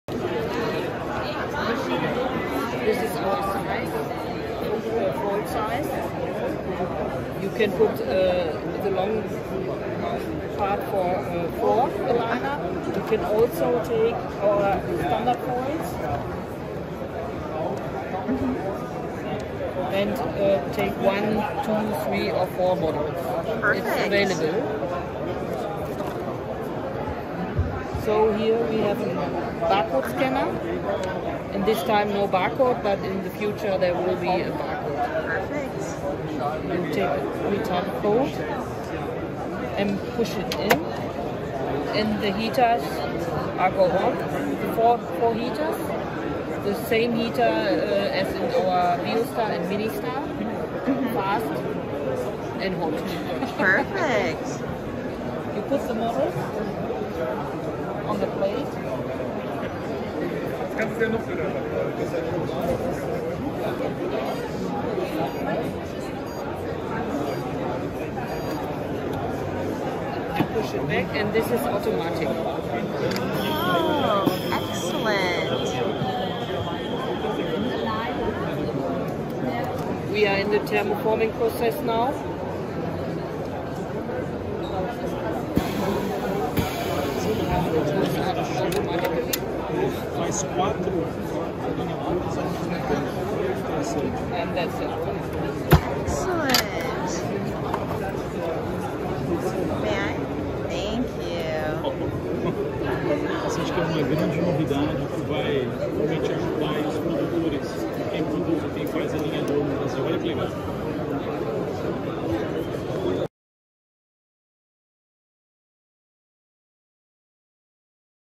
Biostar 4 Thermoforming Clear Aligners
Dr. Melissa Shotell (aka AlignerBee) is checking out the latest innovation at IDS 2023 in Cologne Germany.  Scheu Dental released the Biostar 4 thermoforming machine to pressure form 1-4 clear aligners at a time.  Increasing automation and efficiency in the dental laboratory.  Check out the demonstration of the new Biostar 4.